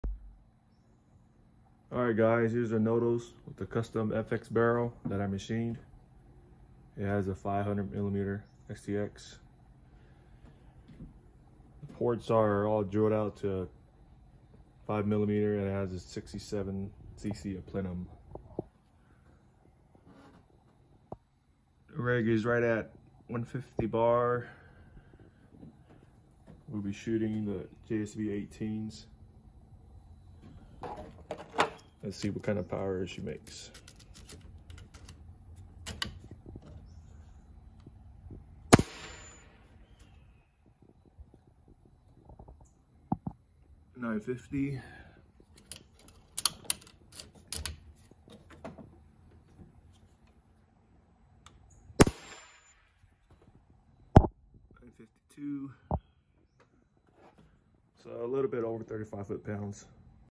35fpe Umarex Notos
Custom Notos with a custom 500mm stx FX barrel. Ports are opened up to 5mm and 67cc of plenum. You can find the product listed on my website.